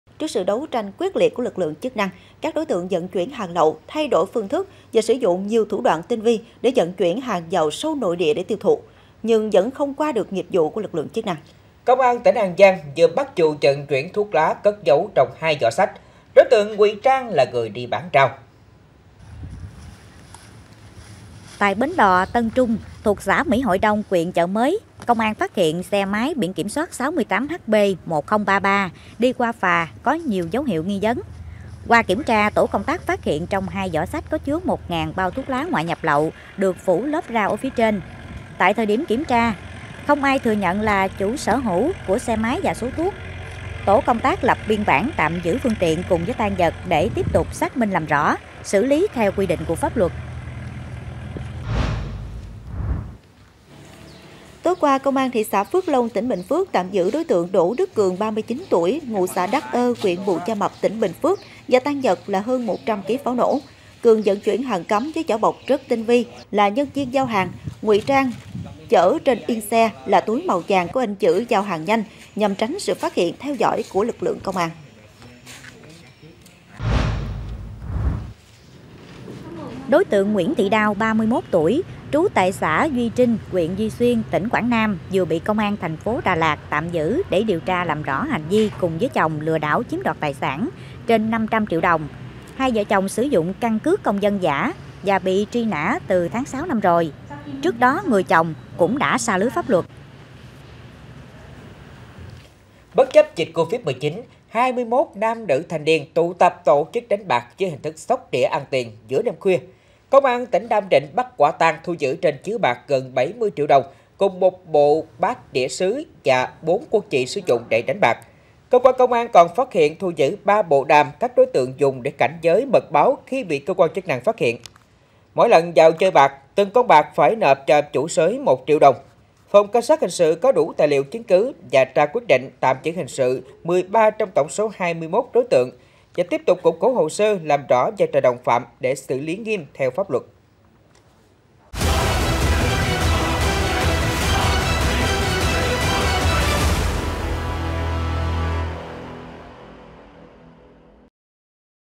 Giấu “hàng” dưới đám rau, đối tượng bỏ của chạy lấy người
Giấu “hàng” dưới đám rau, đối tượng bỏ của chạy lấy người | TIN NÓNG MEKONG

* Zalo: Truyền hình Hậu Giang
* App: Hậu Giang TV
* Điện thoại: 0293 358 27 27